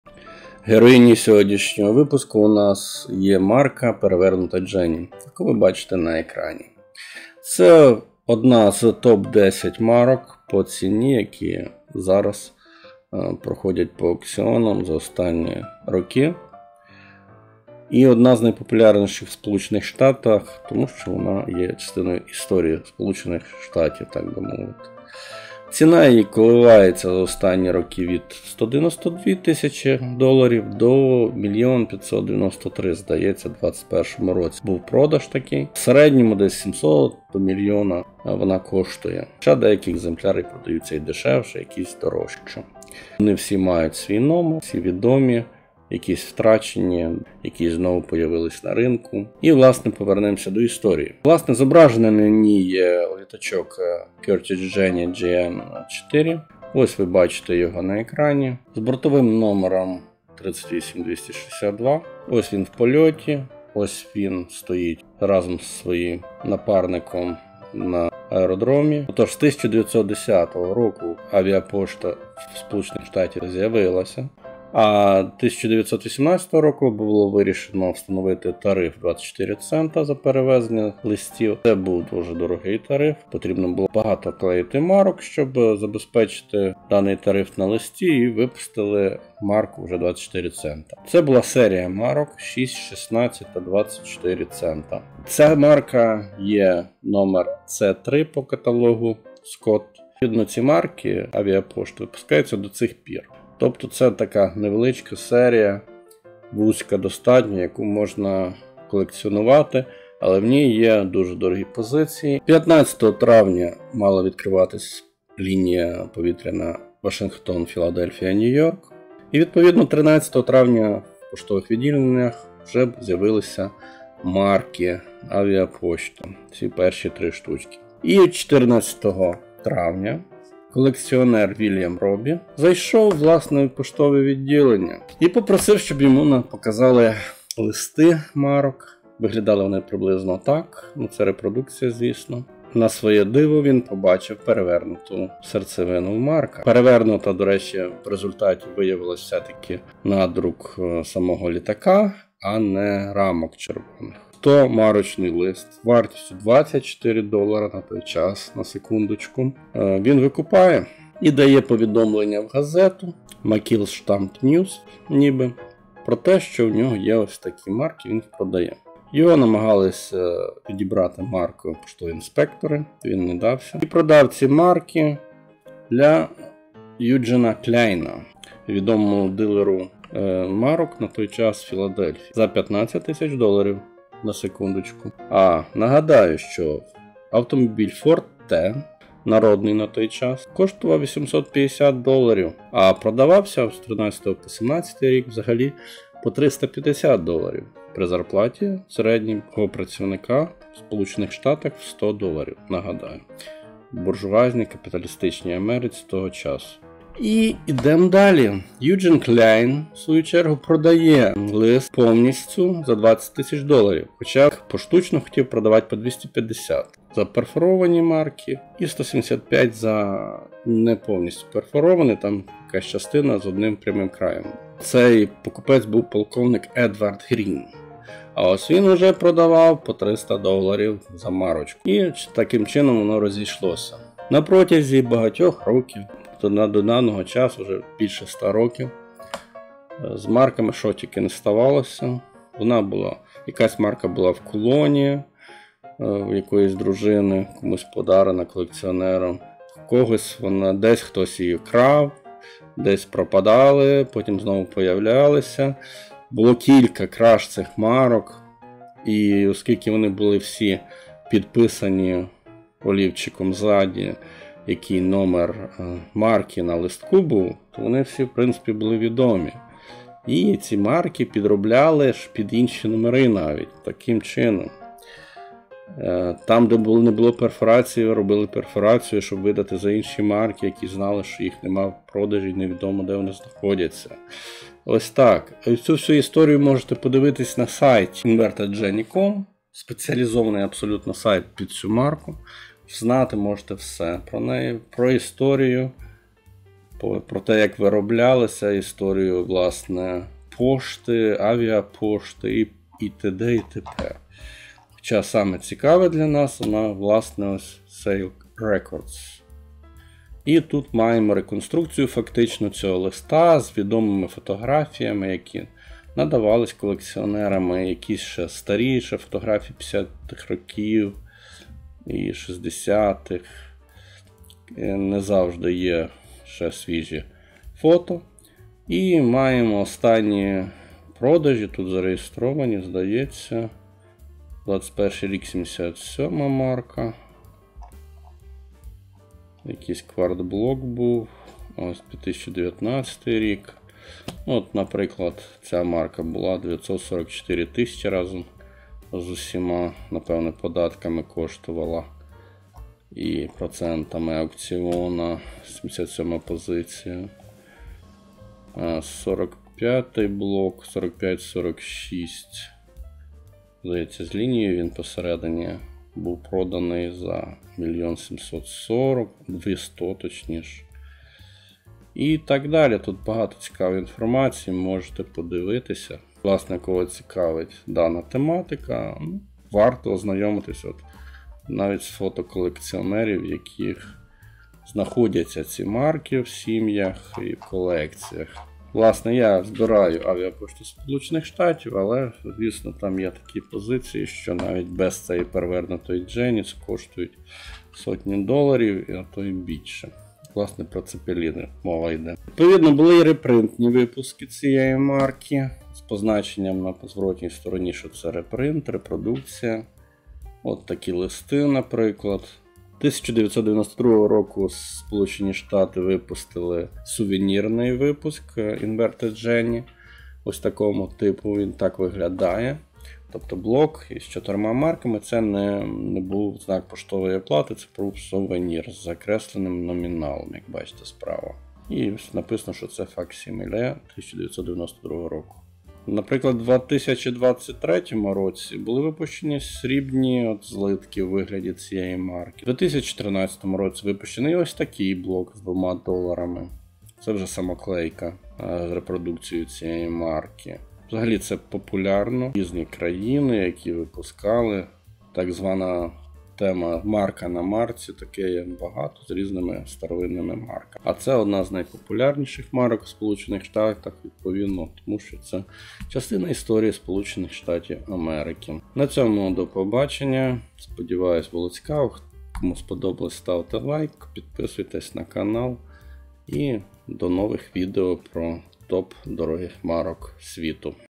INVERTED JENNY 1918 USA - Марка Перевернута Дженні - Авіапошта США
Марка з ТОП 10 дорогих поштових марок світу. Поштова марка авіапошти США 1918 "INVERTED JENNY" (C3 U.S. Air Mail Stamps) - Перевернута Дженні - Ціна марки і її історія